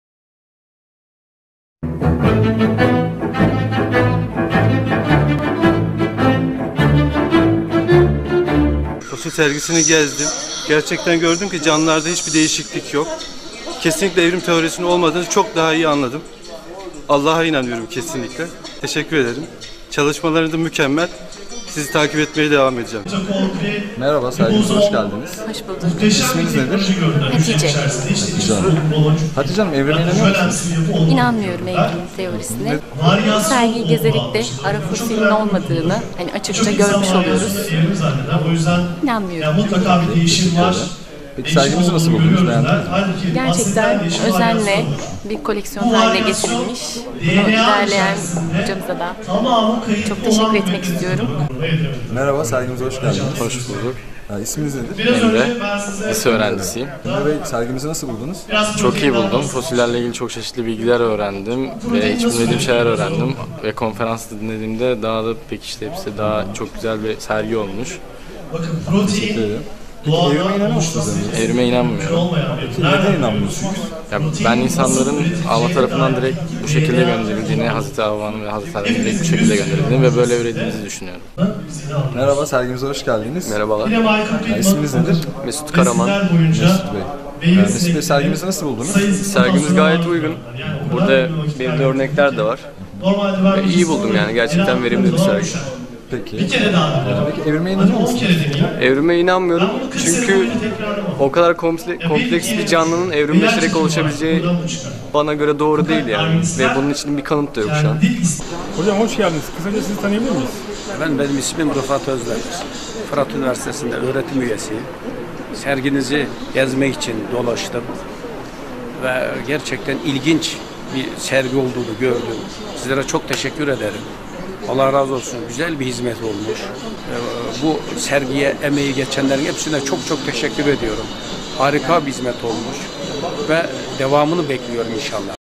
Medyanın Fosil Alerjisi ve yaratılışı anlatanlara duyduğu öfke haberlerle tezahür ediyor
Basının tüm bu kandırma çabalarına rağmen, ￼Halkımız 700 milyondan fazla fosilin yaratılışı ispat ettiğini, ￼Bilimin evrimi yalanladığını biliyor.